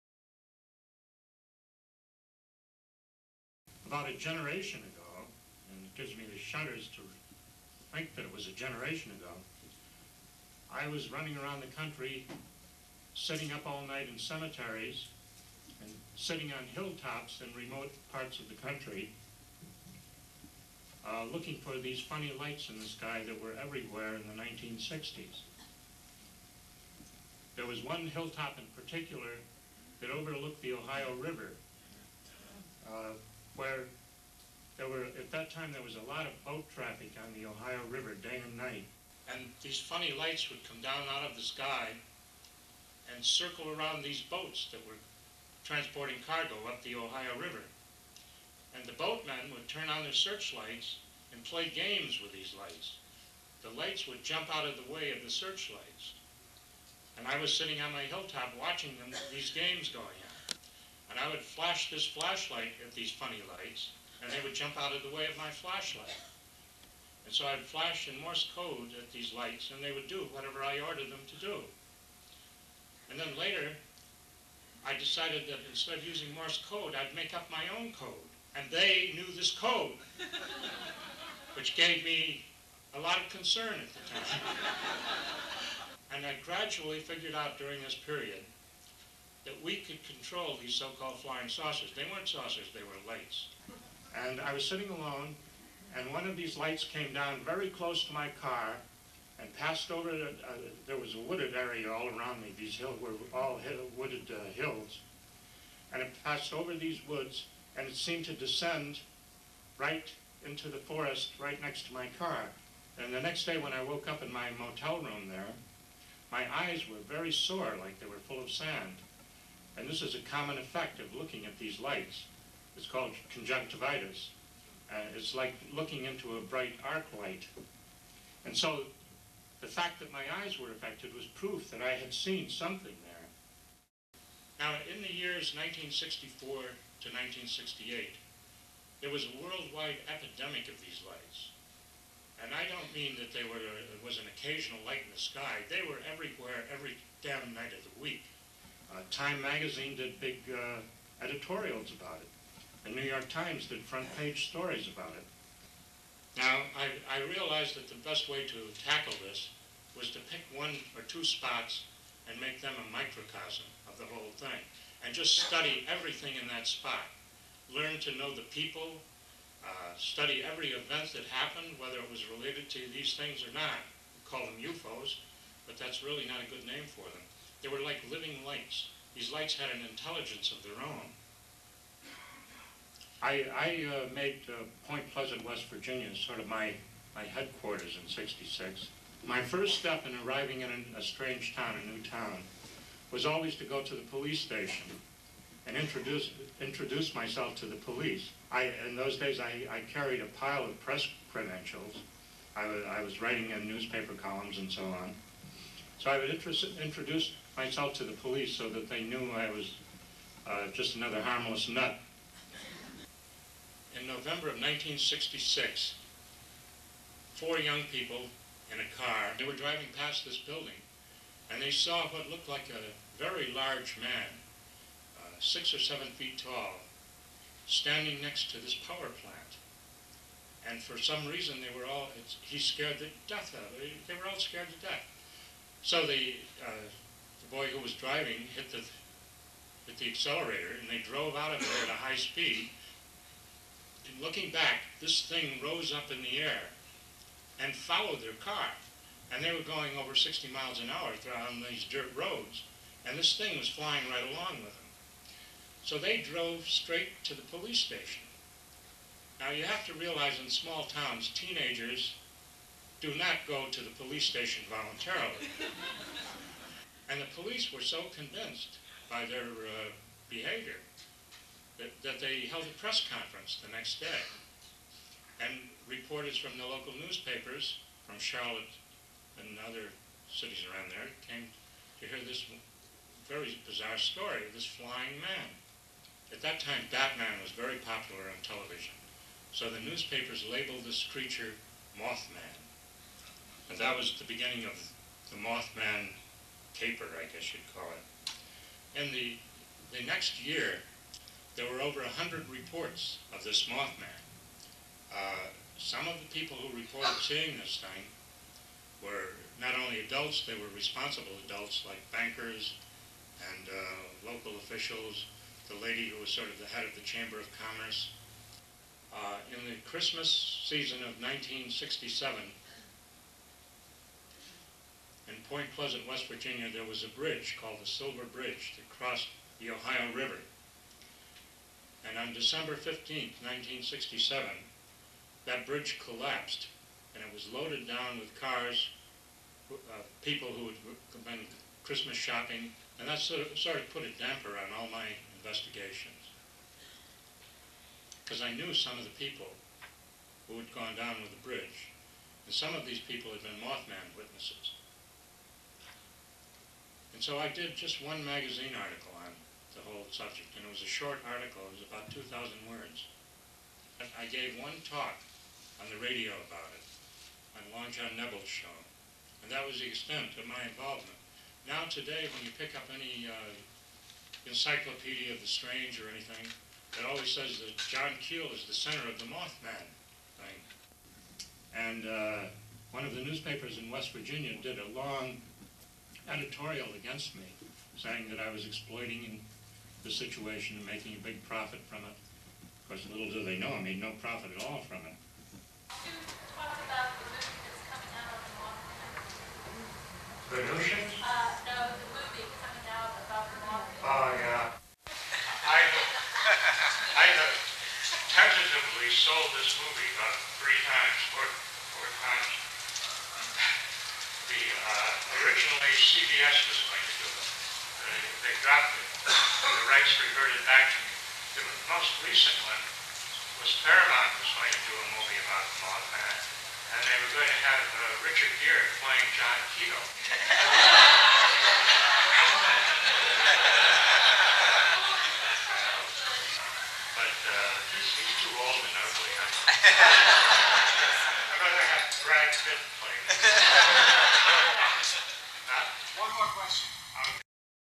John Keel on the Mothman
John Keel discussing "Mothman" from the 1994 and 1999 FortFests. Keel has been a frequent speaker at the International Fortean Organization (INFO) conferences. For more information on the INFO lecture series, go or contact VideoDave2.